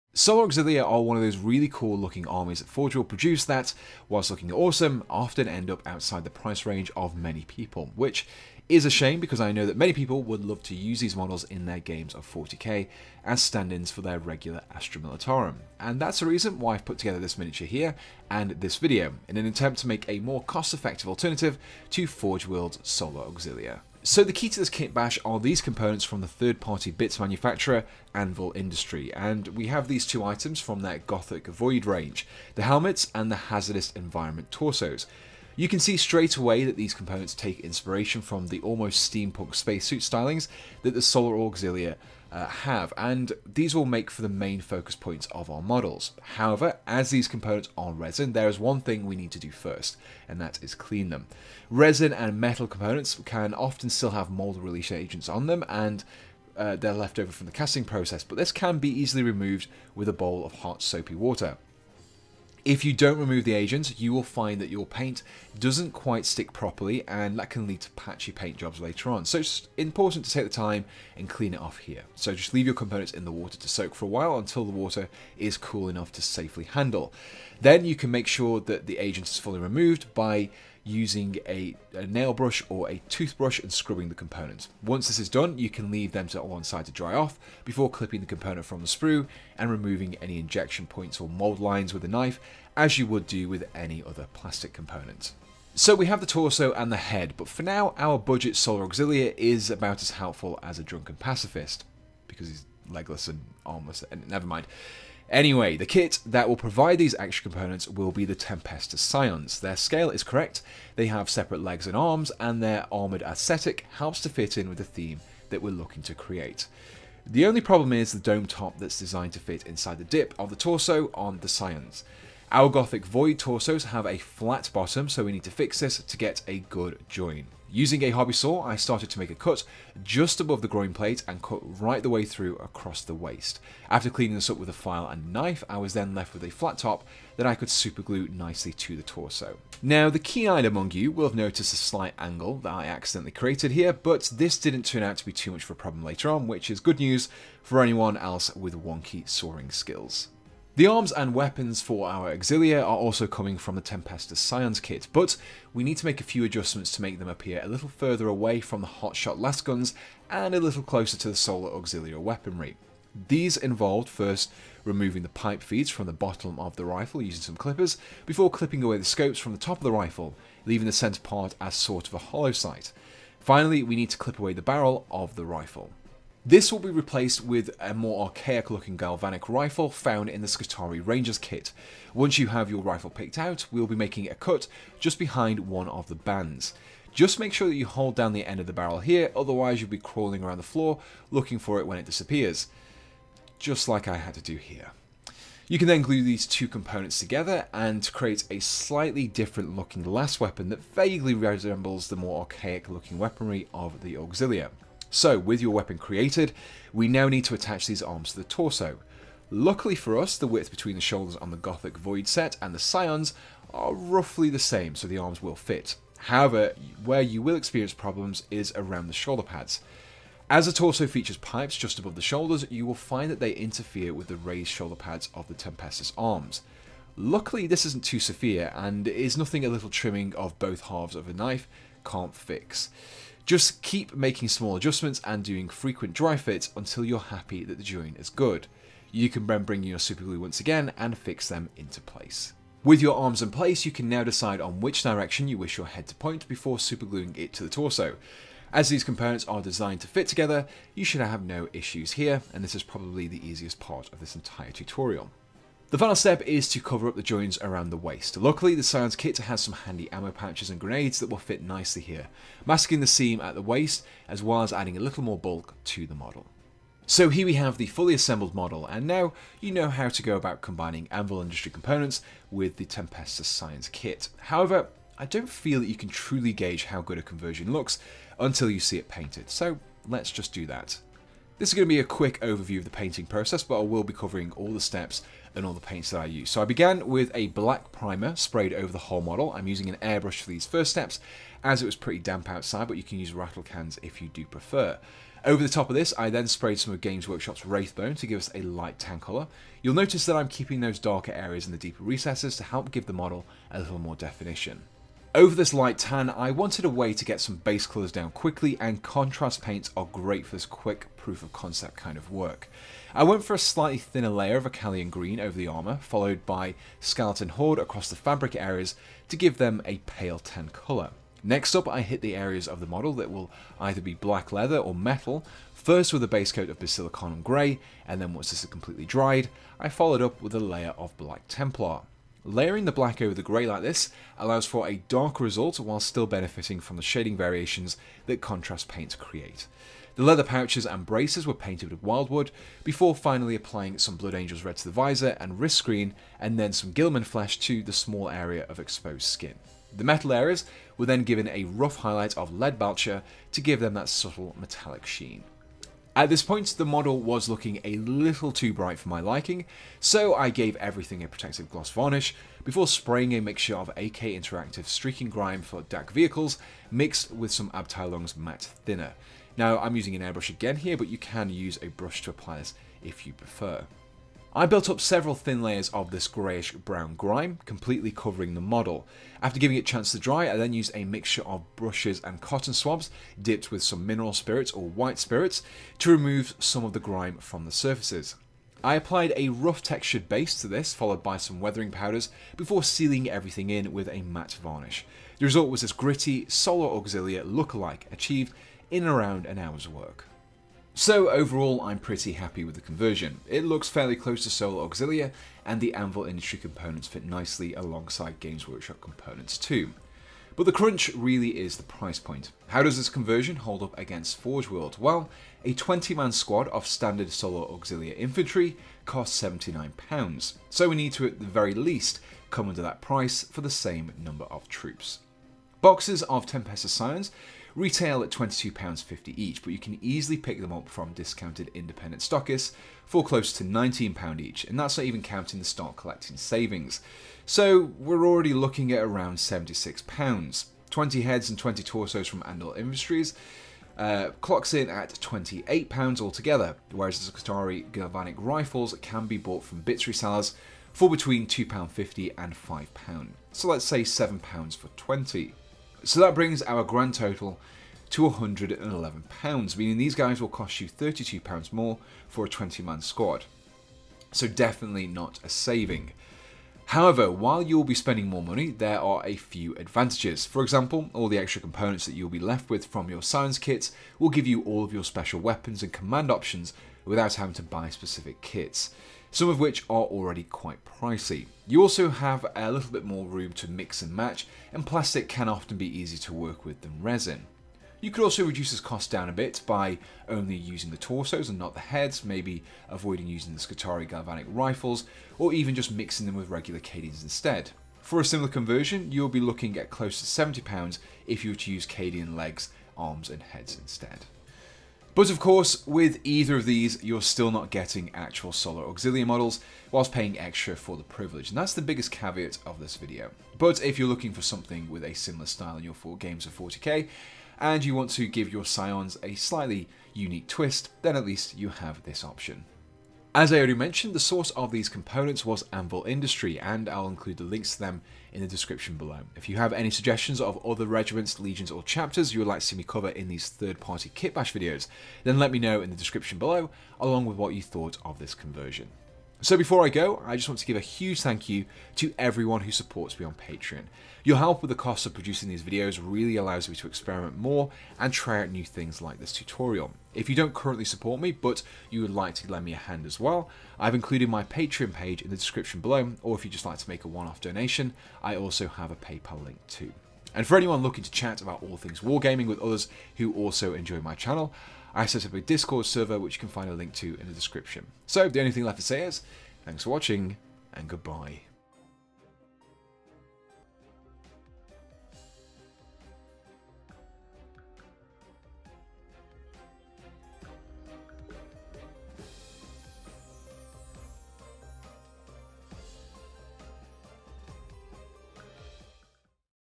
Kit Bashing Solar Auxilia - Third Party Conversions
In this I take a look at kit bashing some Solar Auxila using the some of Anvil Industry's Gothic Void components and some Tempestus Scions.

Equipment Used -
Canon EOS M100